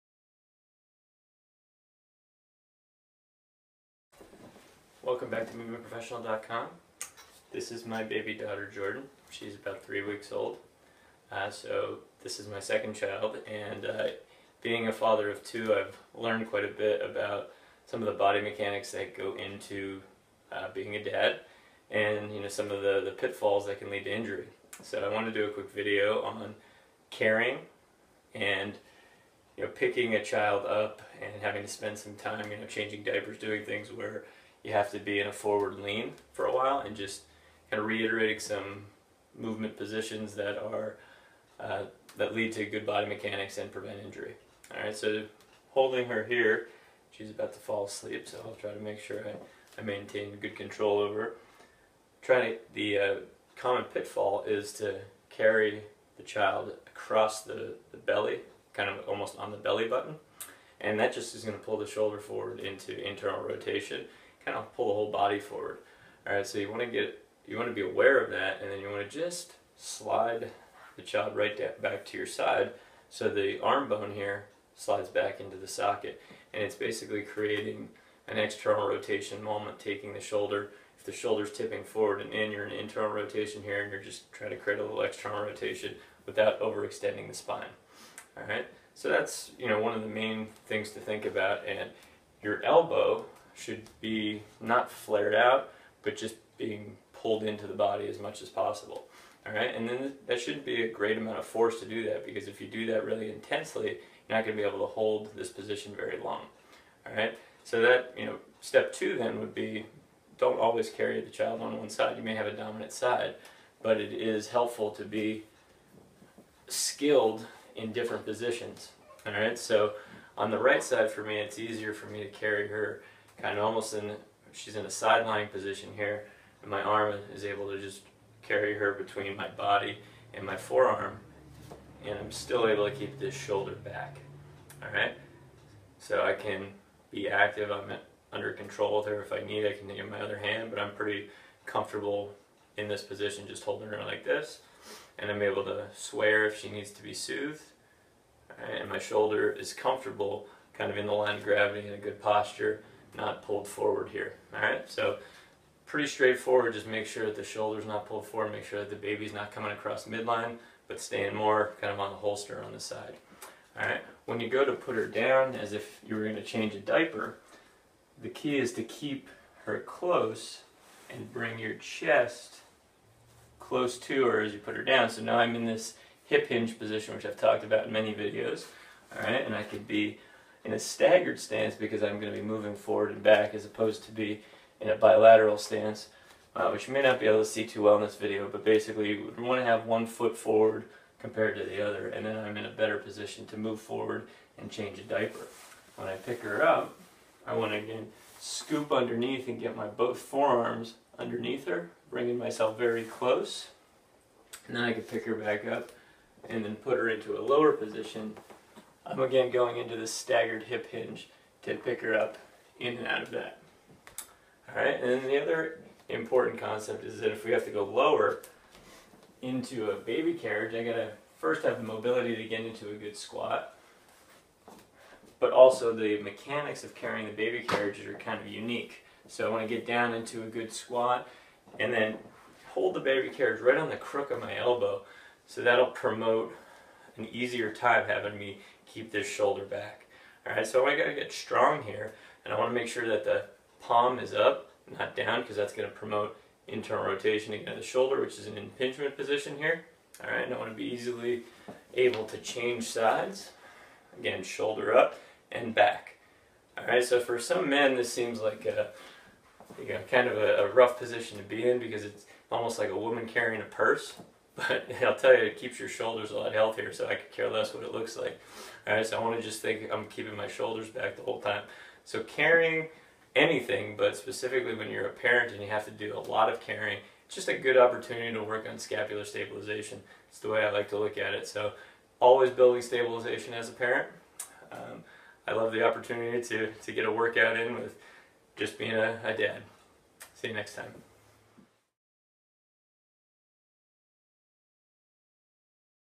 MovementProfessional.com: Body Mechanics for New Parents
Being a parent of two, I have learned a few things regarding child care positions that could easily lead to injury if not corrected. This video is a quick discussion regarding proper positioning while carrying a child both in one's arms or in a car seat/baby carriage, and regarding positions calling for one to lean forward for long periods (i.e. change diapers pick up baby out of crib)